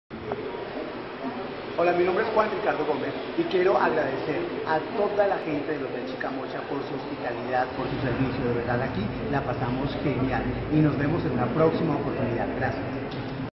Juan Ricardo Gómez recomiendo el Hotel Chicamocha
Juan Ricardo Gómez recomienda al Hotel Chicamocha como la mejor opción hotelera en Bucaramanga. Visitamos.